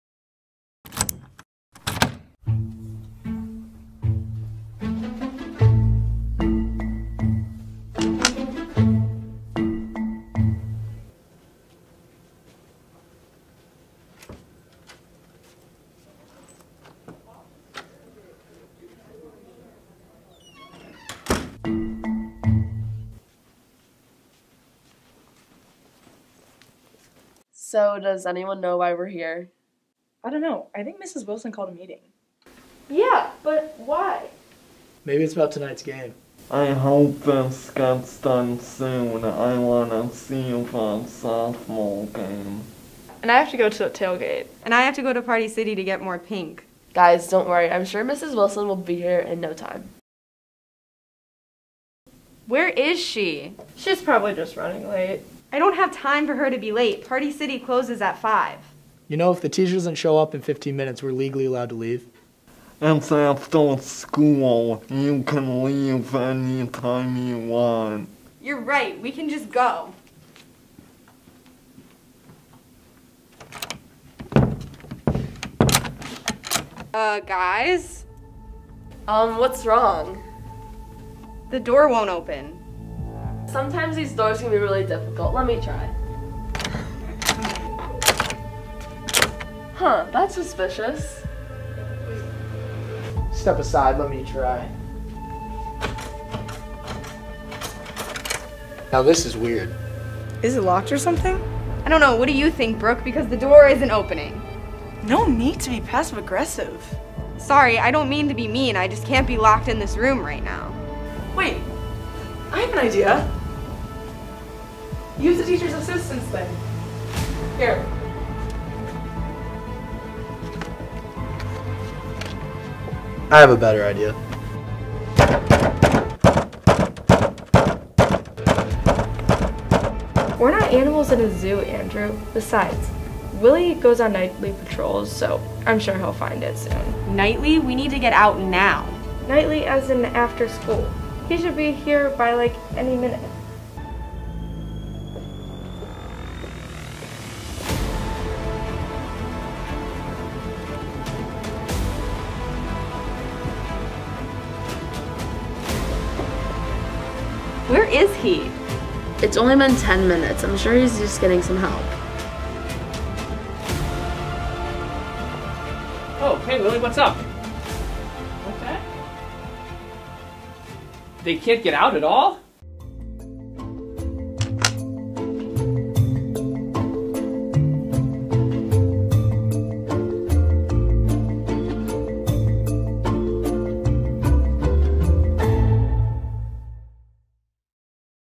Locked In- Part 1 Neuqua Valley Spirit Week 2021
1st episode of Locked In: The Media Production 3 and 4 Spirit Week 2021 series. Follow on Instragram @nvhslockedin and Twitter @neuquamedia.